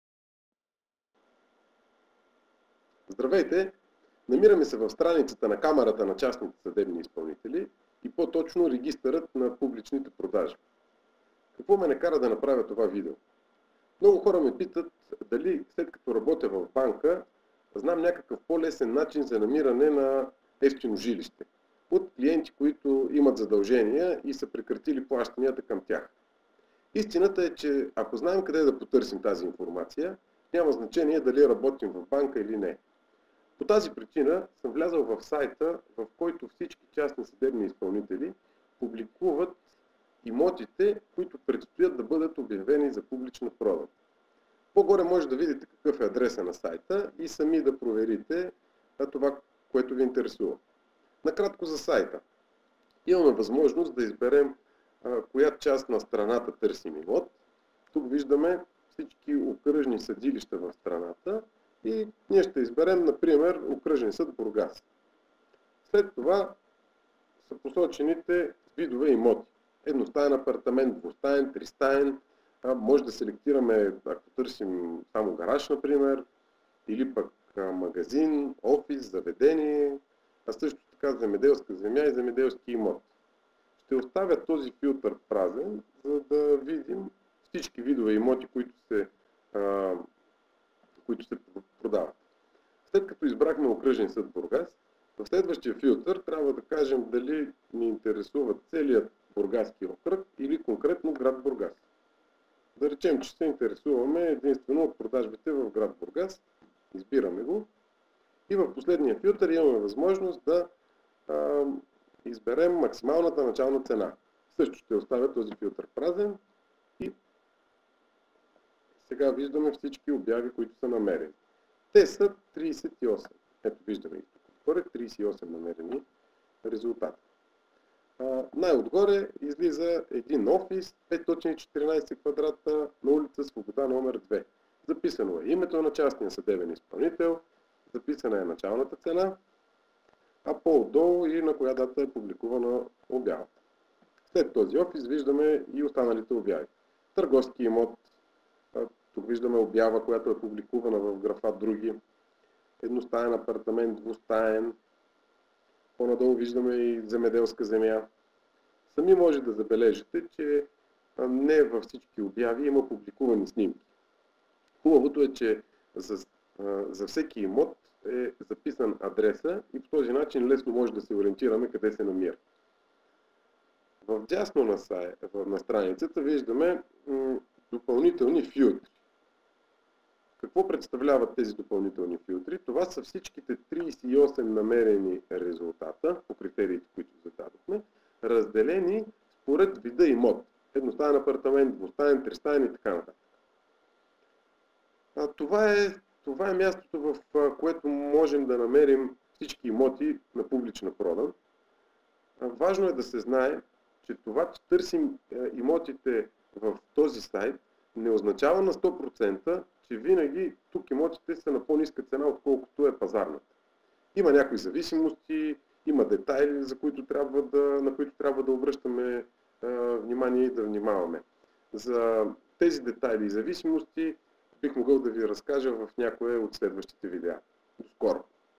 Как да закупя имот на публична продан - търг от частен съдебен изпълнител ЧСИ.
Мястото, на което се публикуват обяви на имоти чрез публична продан. Търг през частен съдебен изпълнител - ЧСИ.